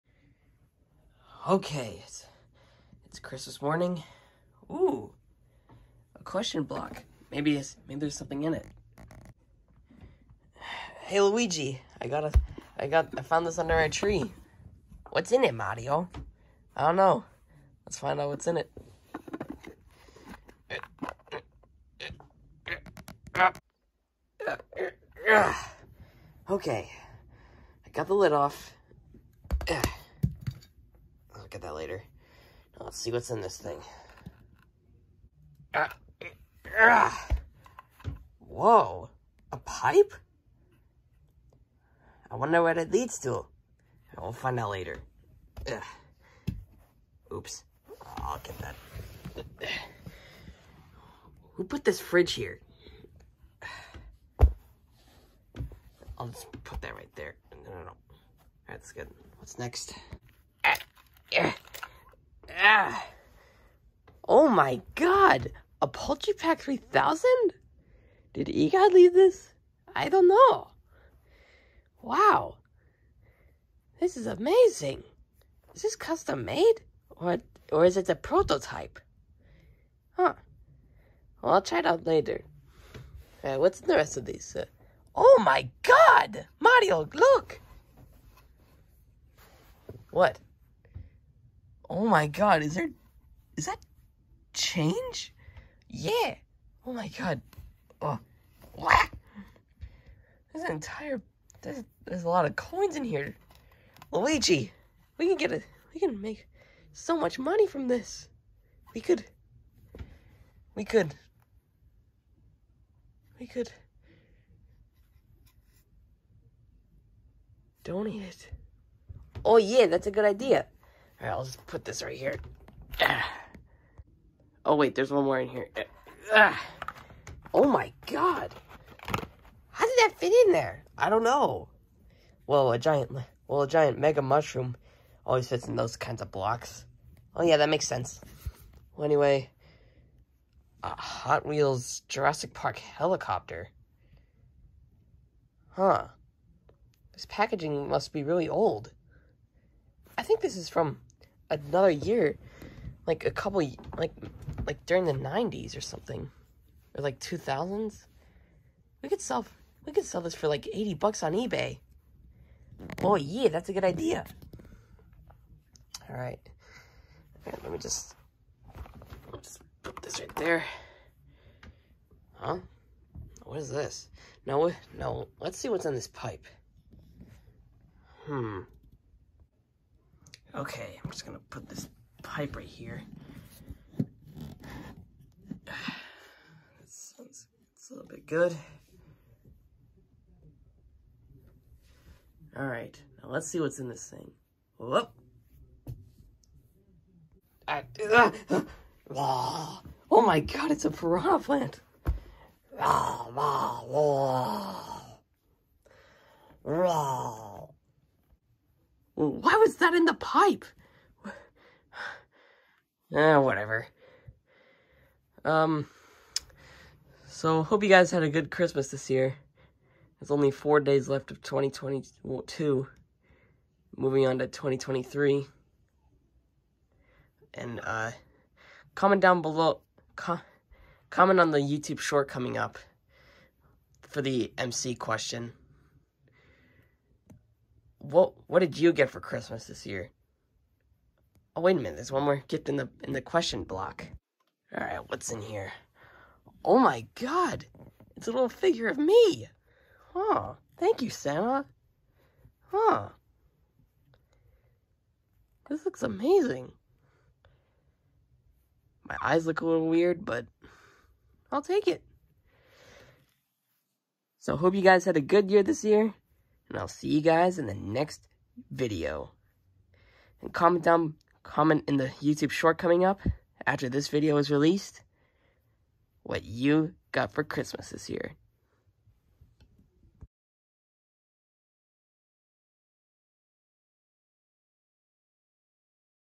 MC short: The Christmas question block
Mario and Luigi got a red question block for Christmas and what was in there was incredible.